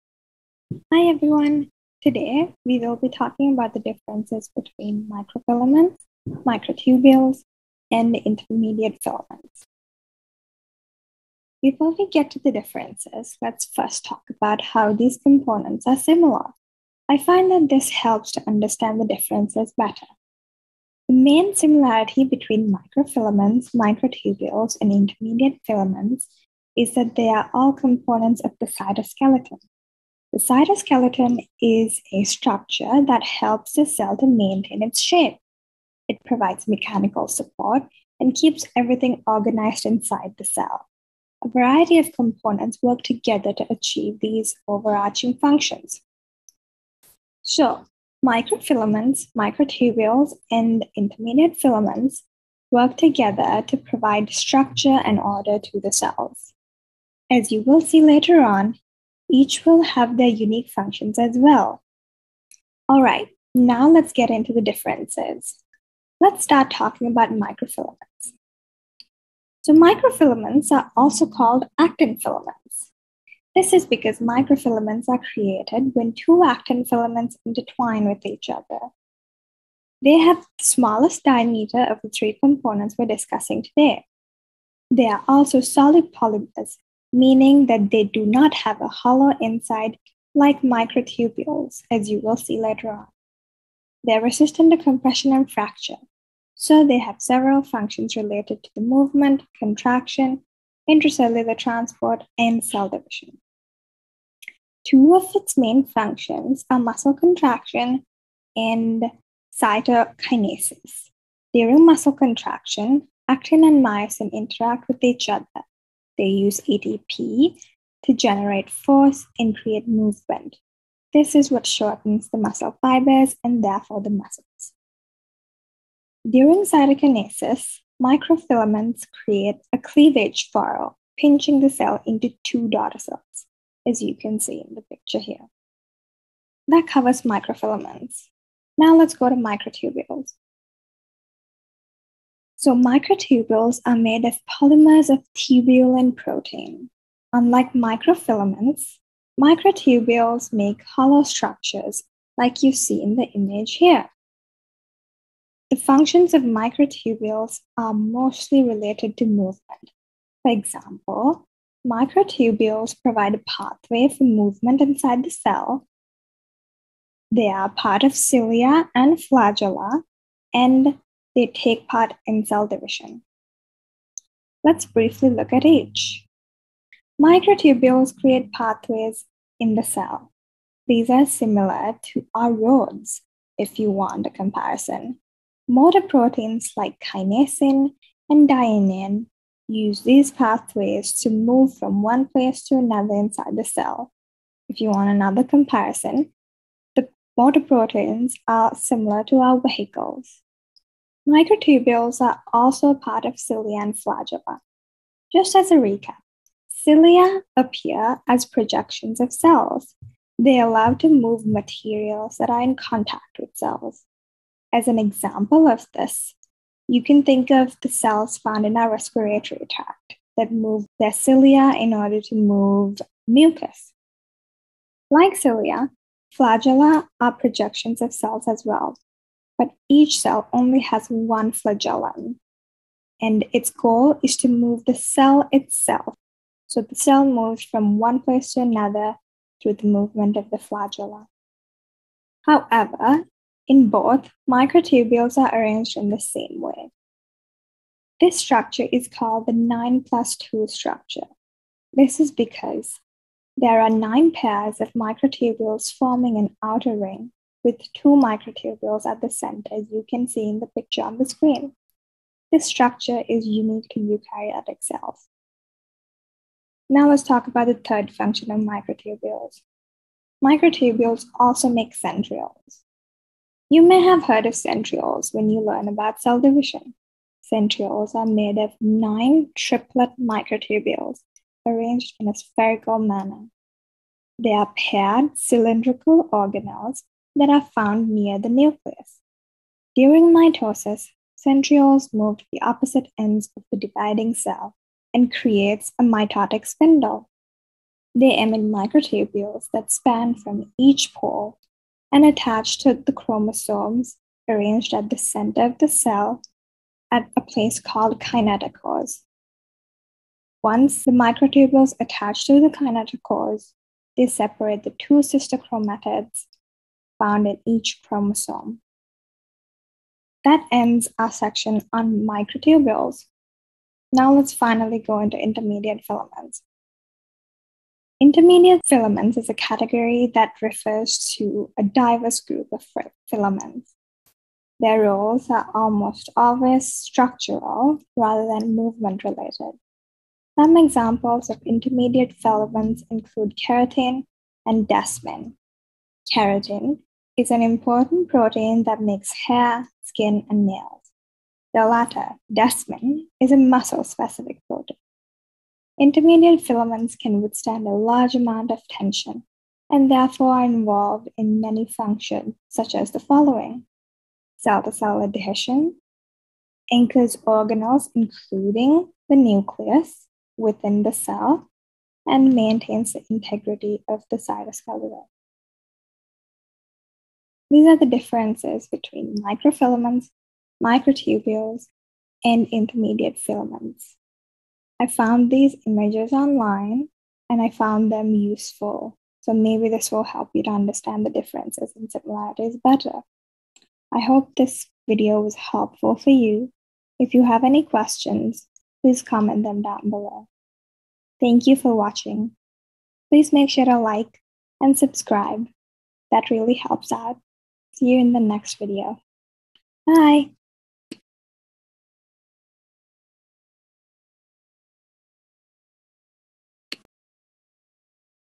Differences Between Microfilaments, Microtubules and Intermediate Filaments | MCAT BIOLOGY
Today we will talk about the similarities and differences between Microfilaments, Microtubules and Intermediate Filaments. Some of the topics I discuss in this video include the following: Cytoskeleton, Actin filaments, Muscle contraction, Cytokinesis, Cilia,  Flagella, 9 + 2 microtubule structure, Centrioles, Mitotic spindle and, Keratin and Desmin.

Timestamps:
0:00 Intro
0:14 Similarities between Microfilaments, Microtubules and Intermediate Filaments
1:08 Microfilaments
1:47 Functions of microfilaments
2:22 Microtubules and their functions
3:21 Cilli and Flagella
4:29 Centrioles
5:29 Intermediate filaments
6:18 Helpful resources
6:35 Outro

I hope this was helpful for you.

References:
Kaplan’s MCAT Biology Review
Biology Libretexts